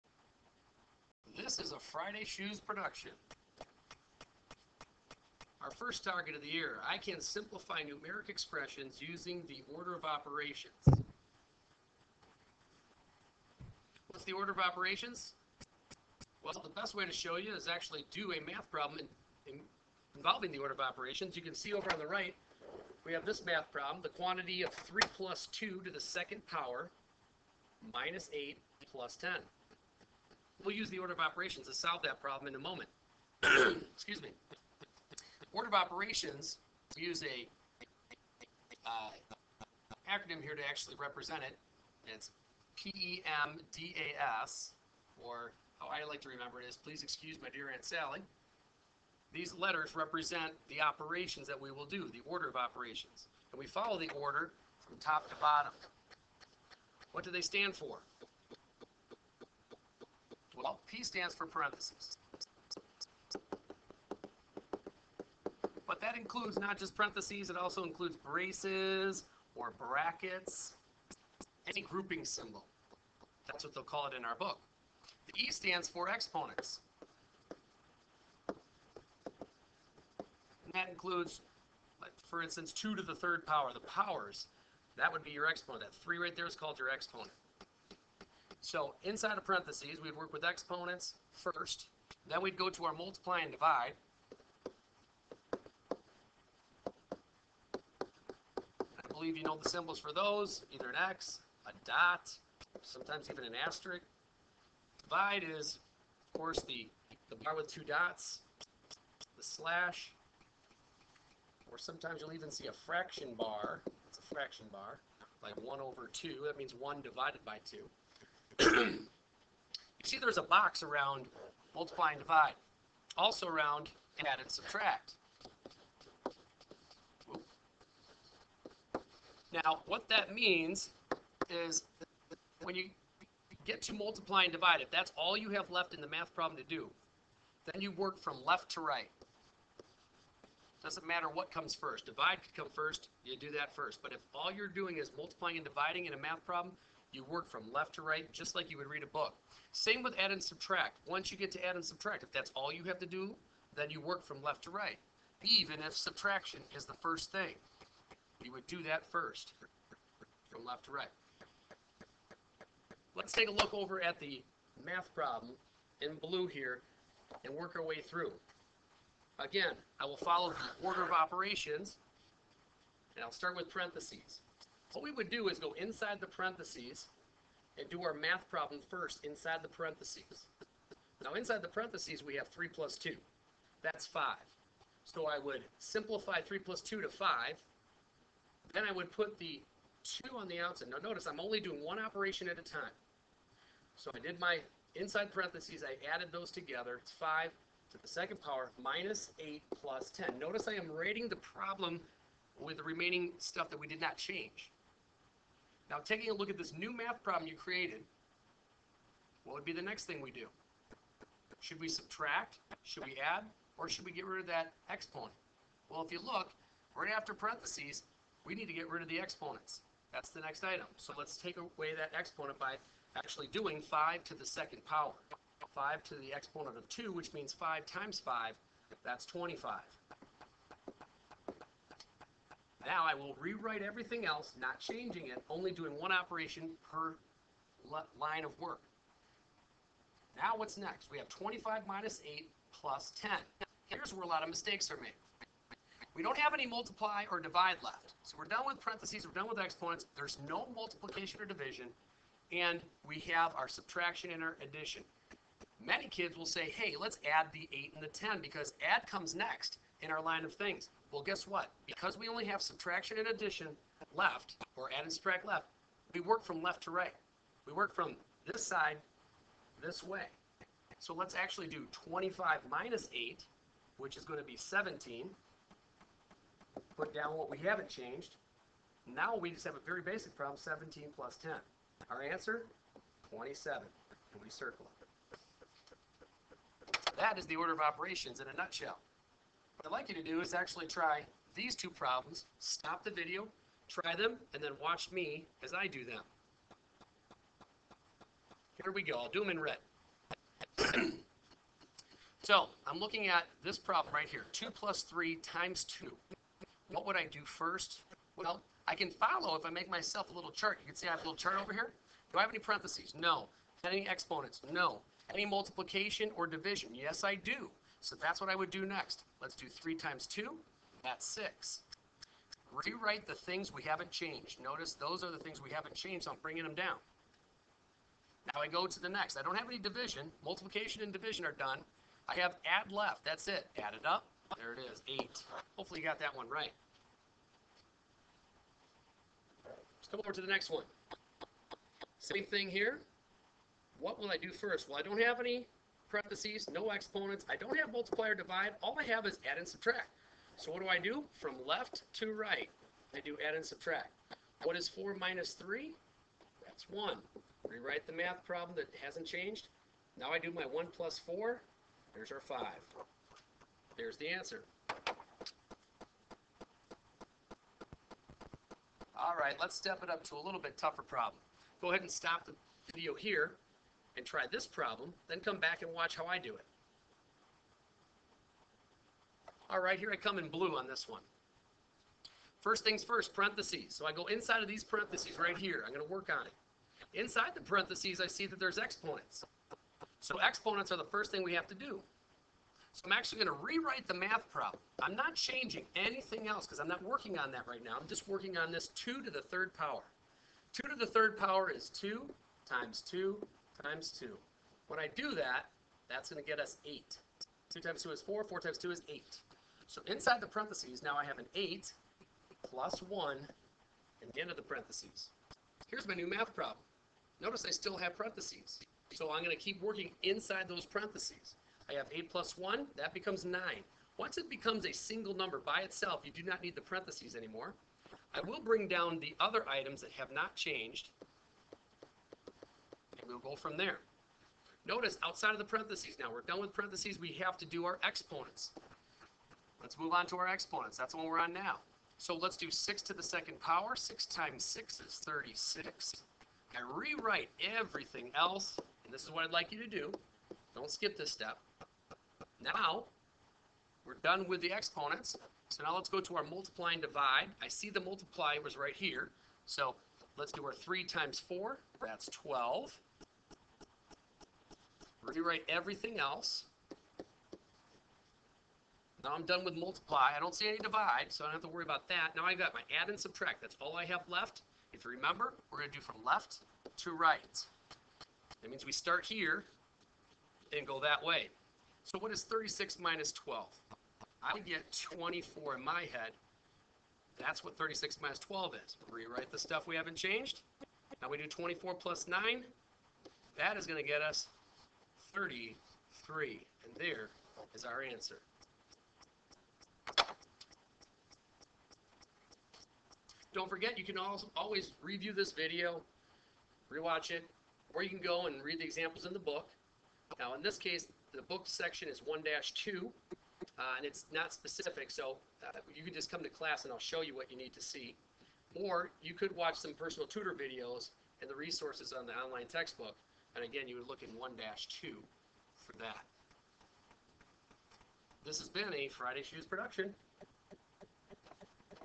Lesson 1-1 Simplifying Numeric Expressions
How to use the order of operations to simplify numeric expressions.